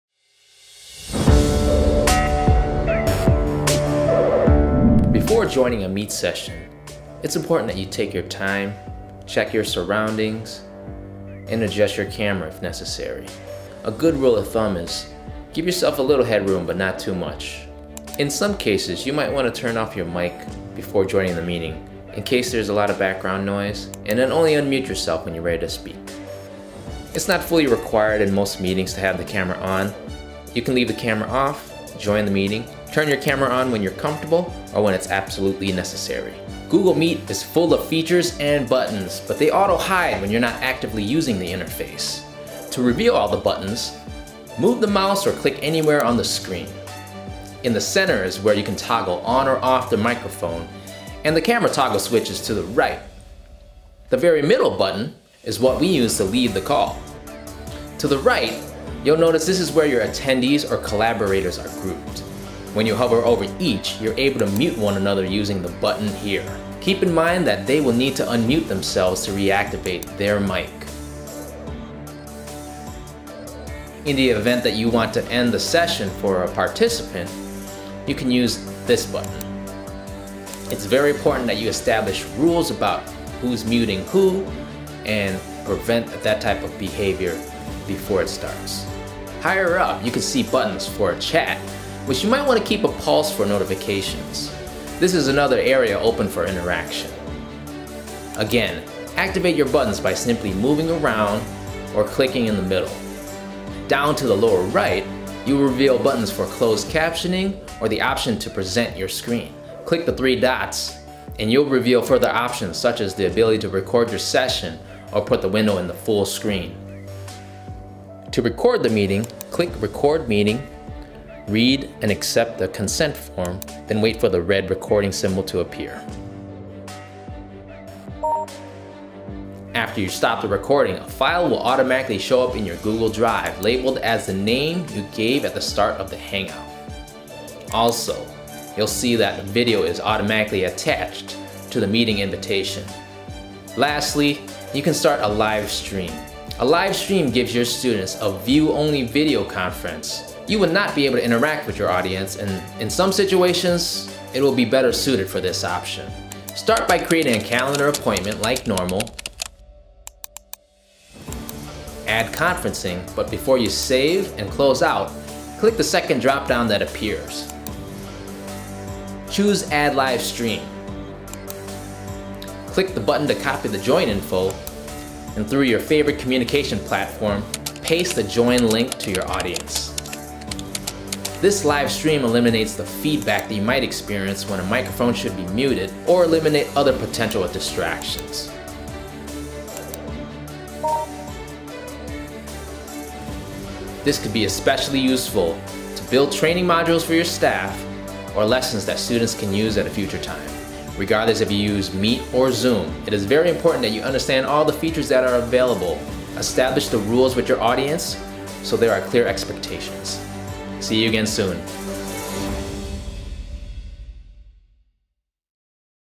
TECH UPDATES: EP37 - Google Meet Tools and Live Stream
Hangouts have hidden buttons so it is important that you learn how to reveal them.  Also, check out if the live stream option will work for your application(s).  We can't stress how important it is to try out all of the options and features to see what works best.

10 GSuite tips to work with remote teams: If your preference is Zoom, here are the ready-made tutorials that will help you get started: